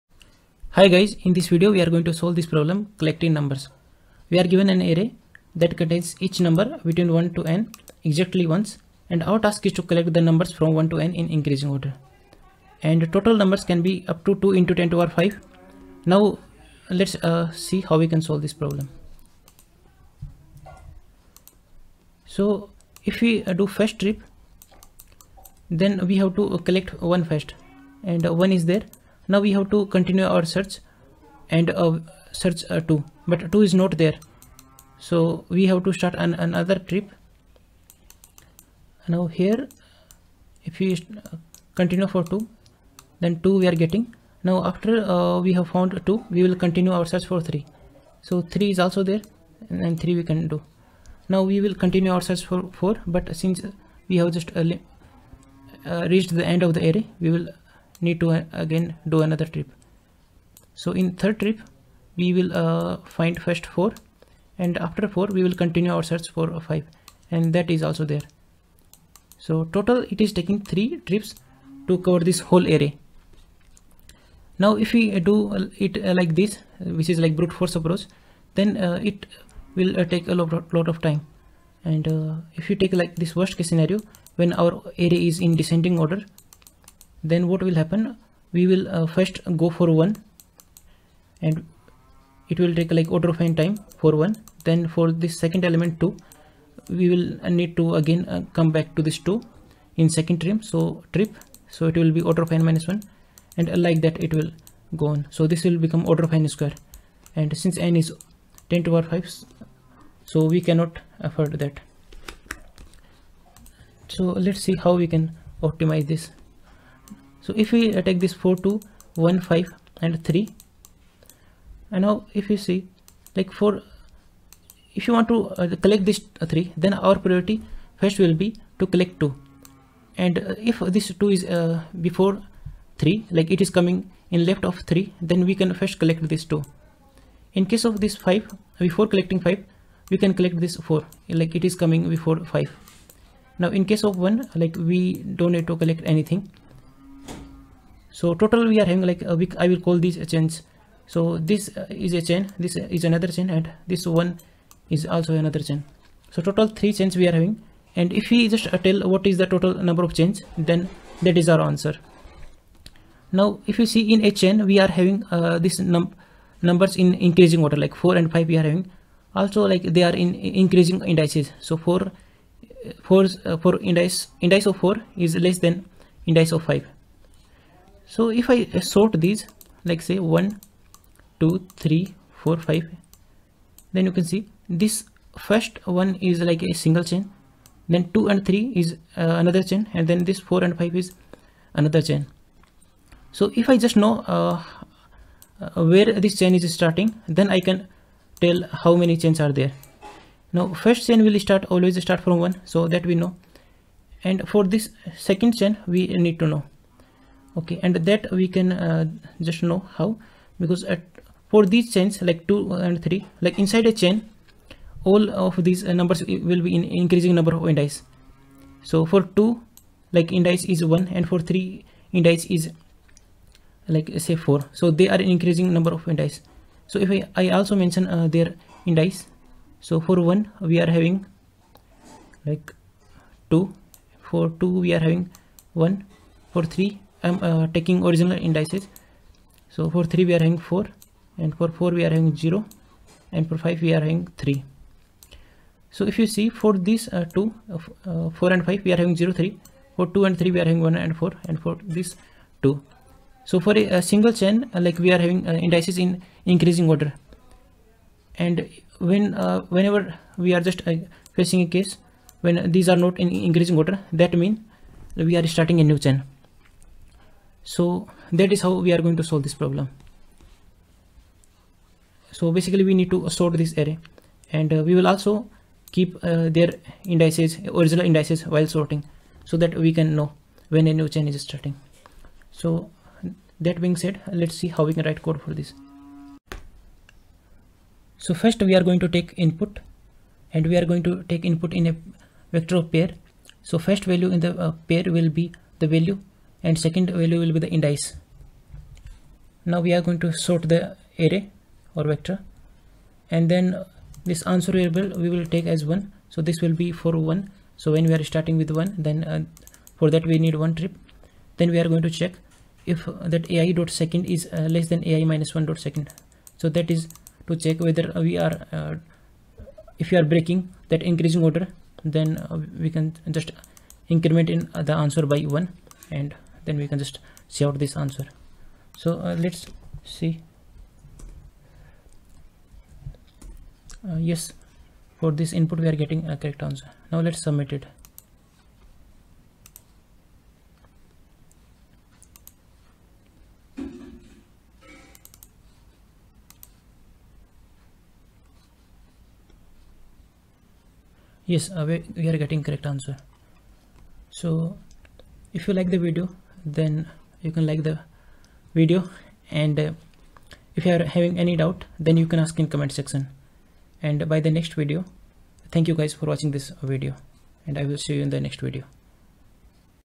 Collecting Numbers | CSES Task 2216 | CSES Sorting and Searching
In this video we have discussed CSES Task 2216
Problem statement::
You are given an array that contains each number between 1…n exactly once. Your task is to collect the numbers from 1 to n in increasing order.
On each round, you go through the array from left to right and collect as many numbers as possible. What will be the total number of rounds?